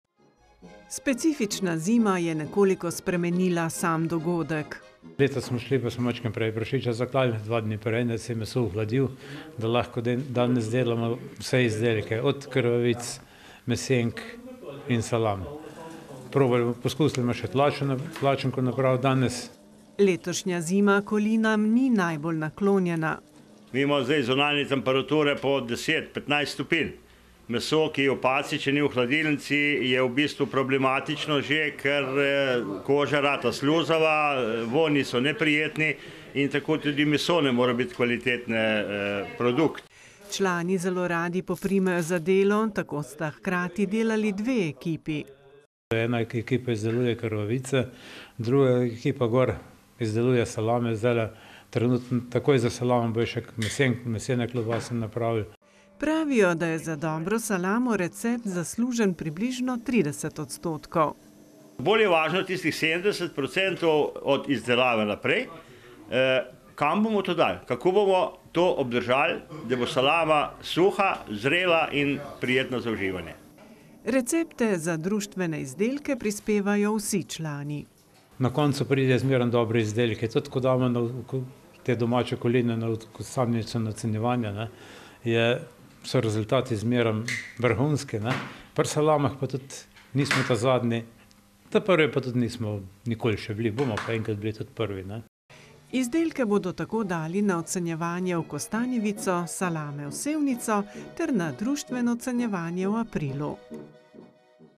Desete društvene koline
LESKOVEC PRI KRŠKEM - Člani Društva ljubiteljev suhomesnatih dobrot Leskovc pri Krškem so pripravili že desete tradicionalne društvene koline. Letošnji gostitelj izdelave dobrot je bil Milan Janežič iz Škocjana.